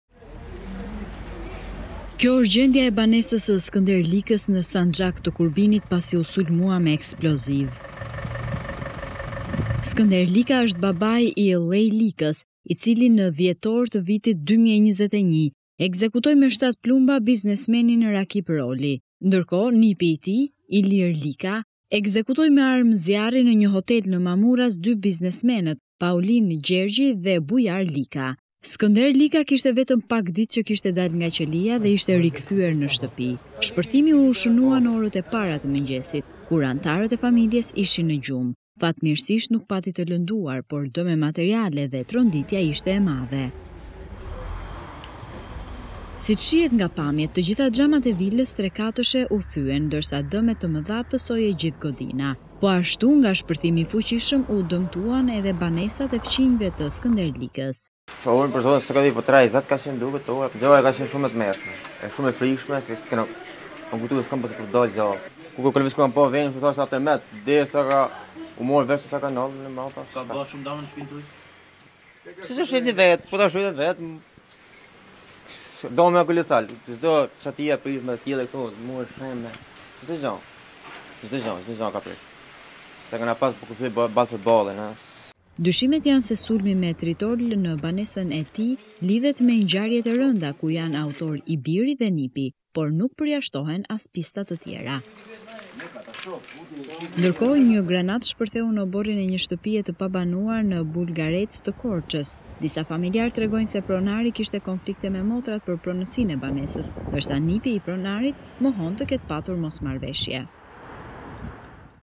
I vihet tritol banesës së Skënder Likës, djali i tij në 2021 u akuzua se vrau biznesmenin Rakip Roli
Banesa e Skënder Likës, djali dhe nipi i të cilit janë në burg të përfshirë në dy krime ku viktima kanë qenë biznesmenë u sulmua me tritol në Sanxhak të Kurbinit.  Viktima nuk pati, por shpërthimii fortë që sipas fqinjëve u ndje si tërmet, ka shkaktuar deme të mëdha materiale. Në Korcë një banesë u sulmua me granatë
Kjo është gjendja e banesës së Skënder Likës në Sanxhak të Kurbinit pasi u sulmua me eksploziv

Skënder Lika, është babai i Ilaj Likës, i cili në dhjetor të vitit 2021 ekzekutoi me 7 plumba biznesmenit Rakip Roli.

Ndërkohë nipi i tij, Ilir Lika ekzekutoi me armë zjarri në një hotel në Mamurras dy biznesmenet, Paulin Gjergji dhe Bujar Lika.

Ai kishte vetëm pak kohë që kishte dalë nga qelia dhe ishte rikthyer në shtëpi.

Shpërthimi  u shënua në orët e para të mëngjesit kur anëtarët e familjes ishin në gjumë.

Fatmirësisht nuk pati të lënduar, por dëmet materiale dhe tronditja ishin e madhe.

Siç shihet nga pamjet, të gjitha xhamat e vilës tre katëshe u thyen ndërsa dëmtime të mëdha pësoi e gjithë godina.

Po ashtu nga shpërthimi i fuqishëm u dëmtuan edhe banesat e fqinjëve të Skënder Likës.

Dyshimet janë se sulmi me tritol në banesën e Skënder Likës, lidhet me ngjarjet e rënda ku janë autorë i biri dhe nipi, por nuk përjashtohen as pista të tjera.

Ndërkohë një një granatë shpërthue në oborrin e një shtëpie të pabanuasr në Bulgarec të Korcës. Disa familjarë tregojnë se pronari kishin konflikte me motrat për pronësinë e banesës.

Ndërsa Nipi i pronarit mohon te kënë pasur mosmarrveshje.